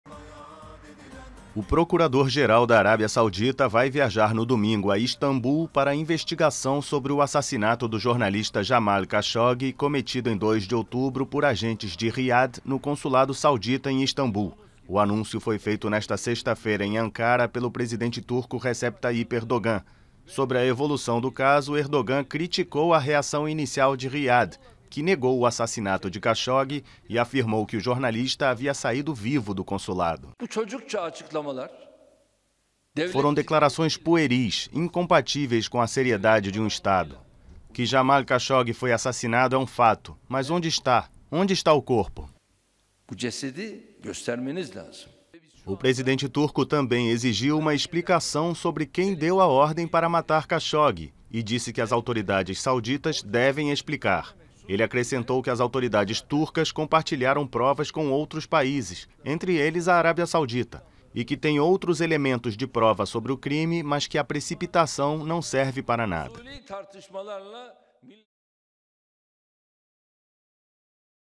Erdogan: ‘onde está o corpo de Karshoggi?’
O presidente turco, Recep Tayyip Erdogan, anunciou nesta sexta-feira que o procurador-geral saudita viajará no domingo a Istambul para a investigação sobre o assassinato de Jamal Khashoggi e afirmou ter mais provas relacionadas ao crime. Ele exigiu que revelem onde está o corpo do jornalista.